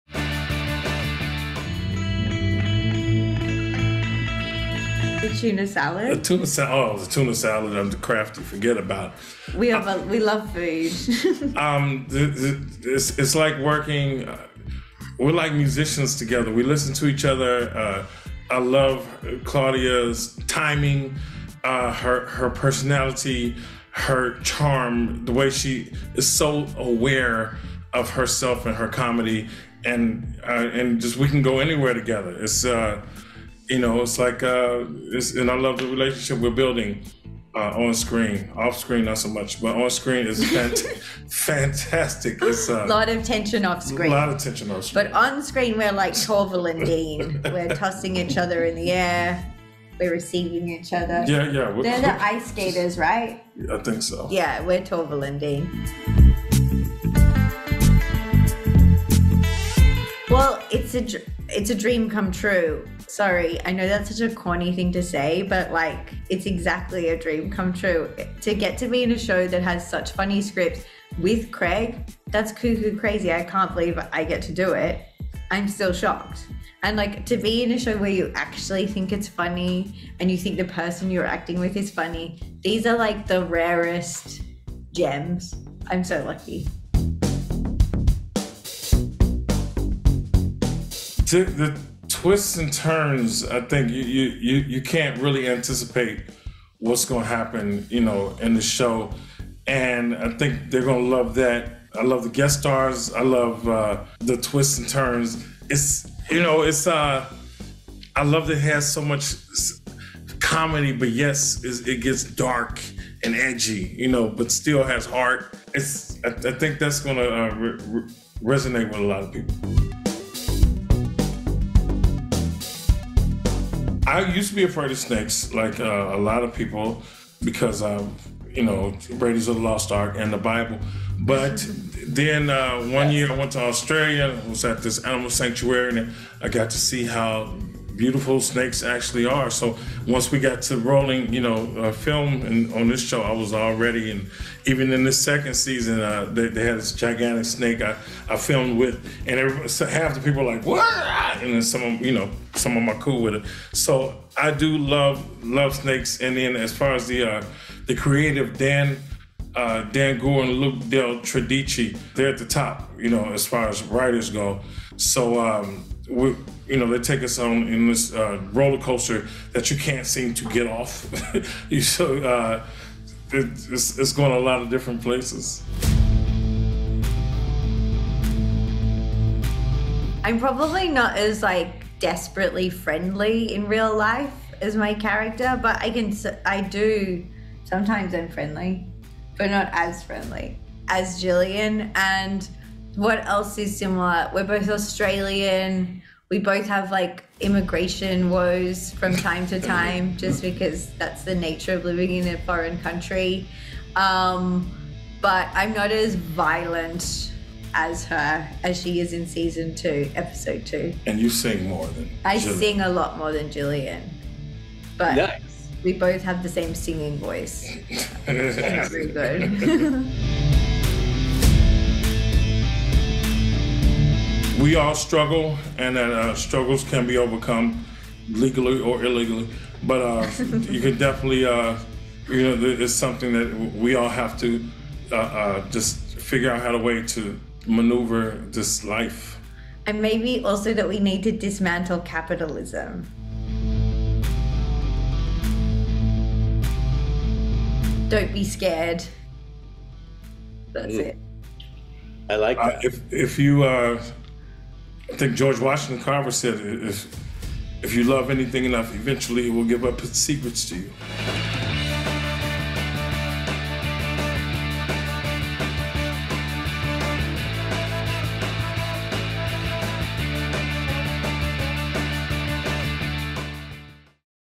Craig Robinson & Claudia O'Doherty Talk Killing It Season 2 | Interview
Killing It stars Craig Robinson and Claudia O'Doherty spoke with MovieWeb about the upcoming second season of the hit show, Killing It.

All 8 episodes of Killing It season 2 premiere August 17, 2023, ONLY on Peacock.

(This interview was conducted prior to the SAG-AFTRA strike)